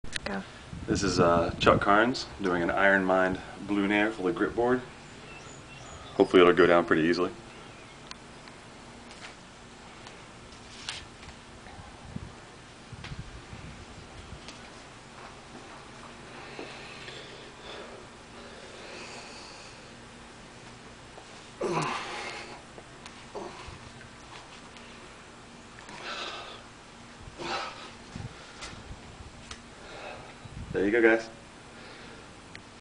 Chuck bends an IM Blue Nail
Chuck Carns bends a piece of 1/4"x6" CRS for the Gripboard.